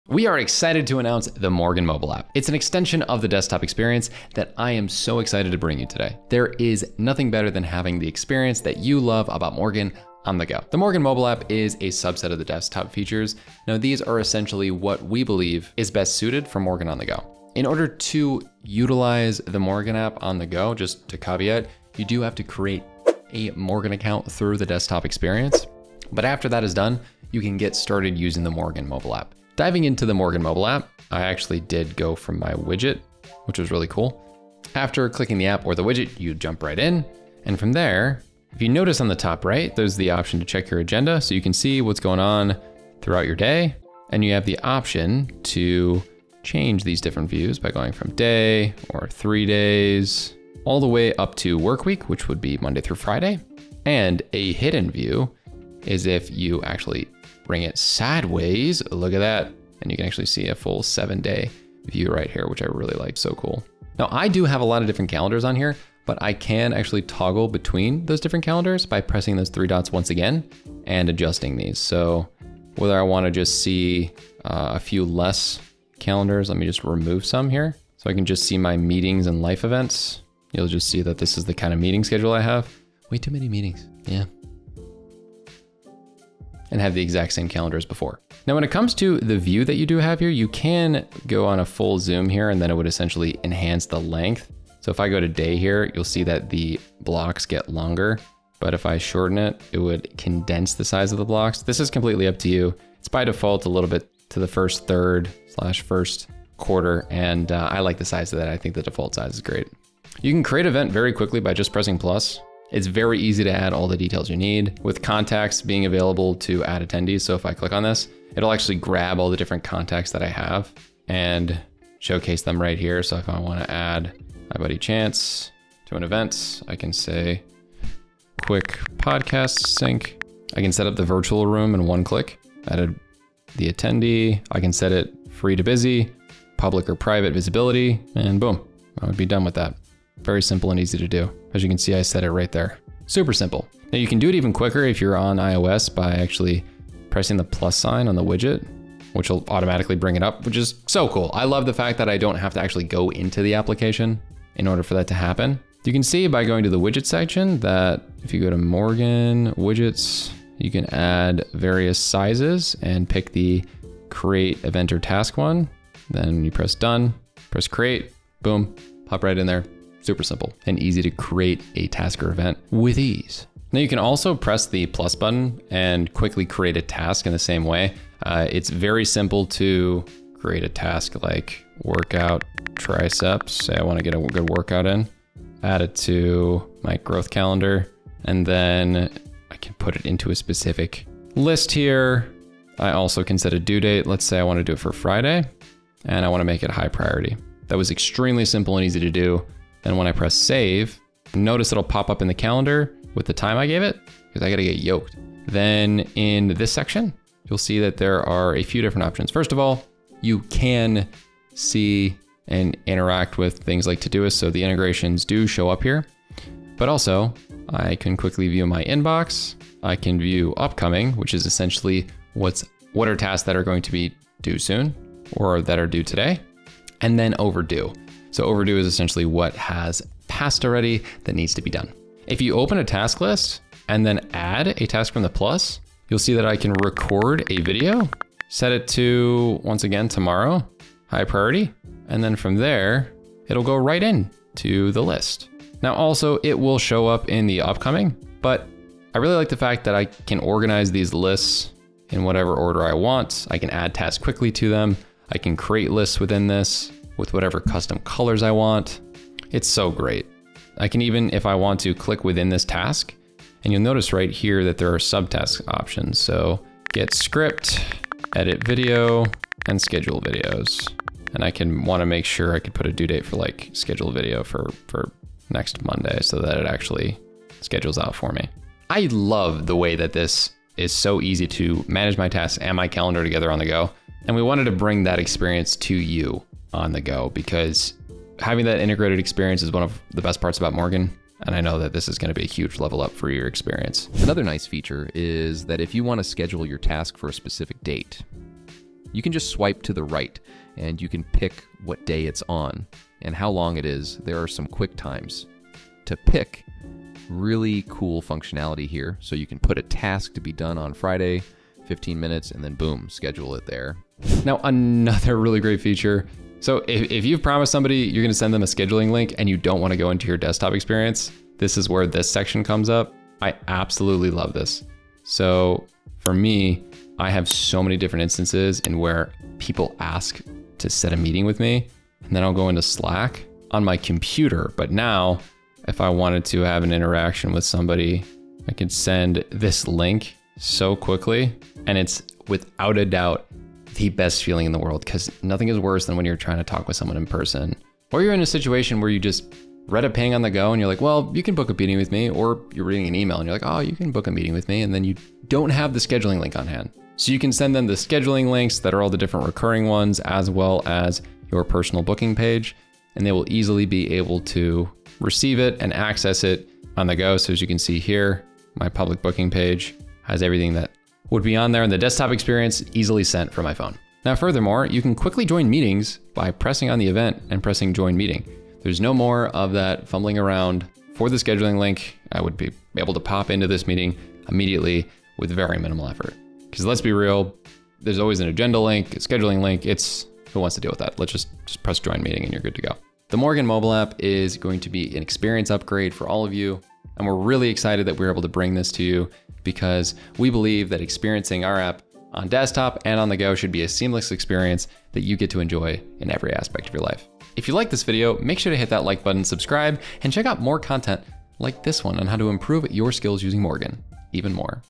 Introducing Morgen's mobile app
Morgen's mobile app is now available with a Morgen Pro subscriptions. Add tasks, create events, join meetings, share your schedule... Morgen's app has many of the key features you depend on in our desktop app.

Here's a preview of some of the top new features and how to put them to use.

Download Morgen from the App or Play stores to get started.